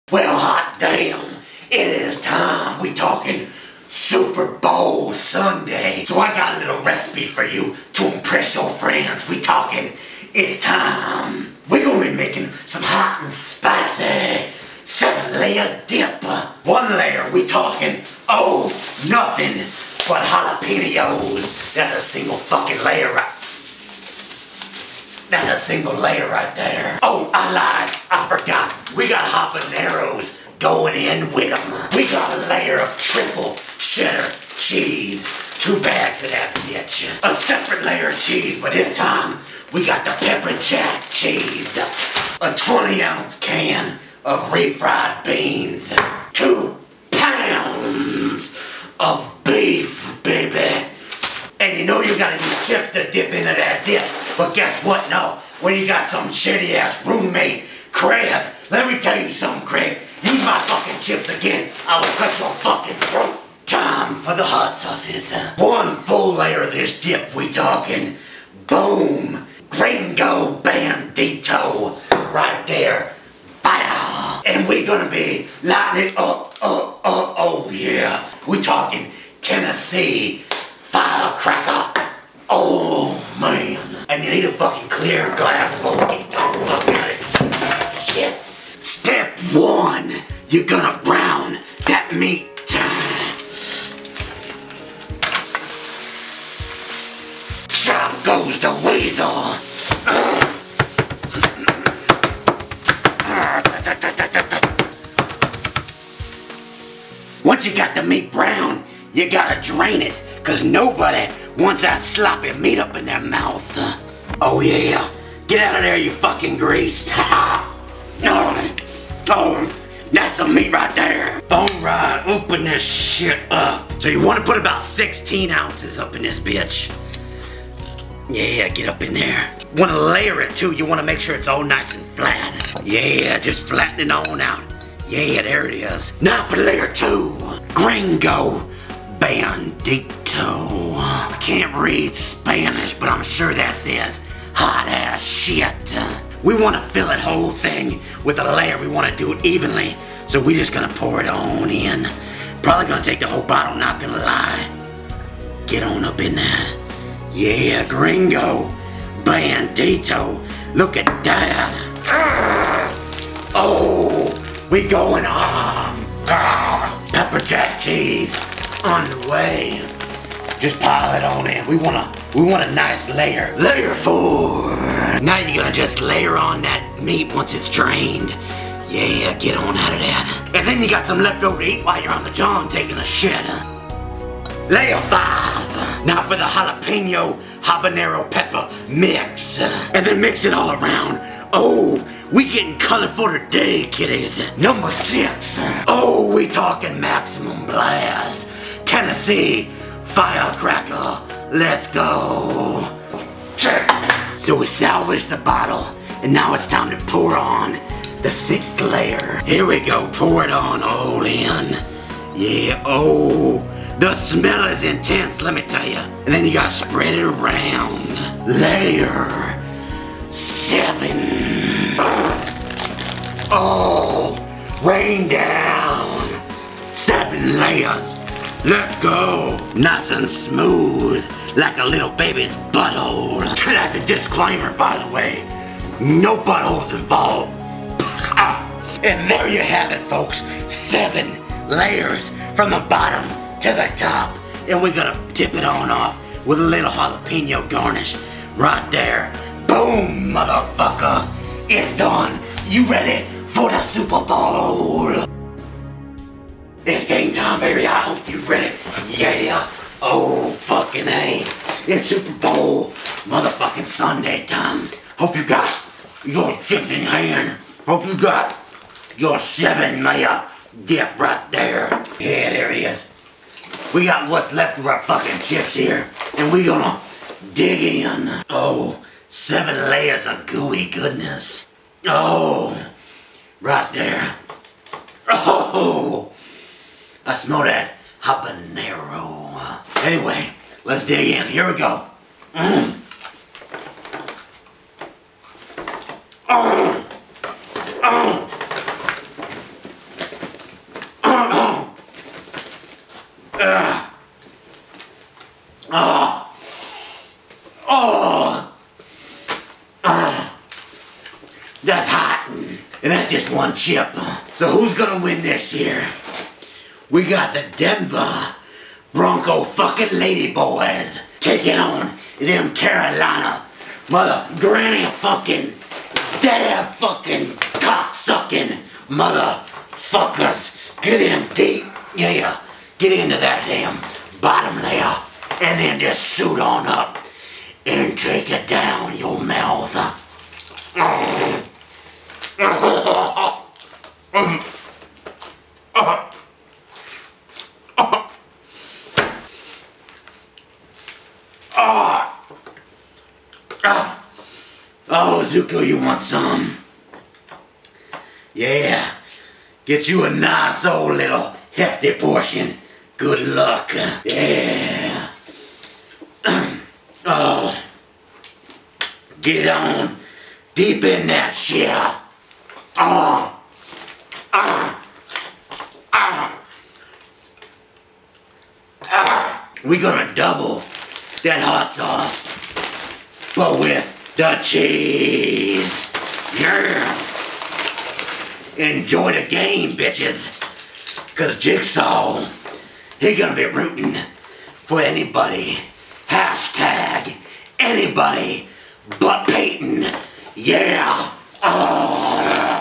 Hot Spicy 7 Layer Dip - Double The Hot Sauce
On this episode of "Double The Hot Sauce" it is Super Bowl Sunday and just like last year Jigsaw has a new dip to show you for your big party! Great ready to feel the burn because we are talking HOT 'N' SPICY 7 LAYER DIP TIME! Enjoy!

Hot sauces used on this episode:
Gringo Bandito Hot Sauce
Tennessee Firecracker

Look, I got an Instagram account!

Oh... and a Twitter!!! Give a follow eh?

DA HELL!?! Facebook too!?!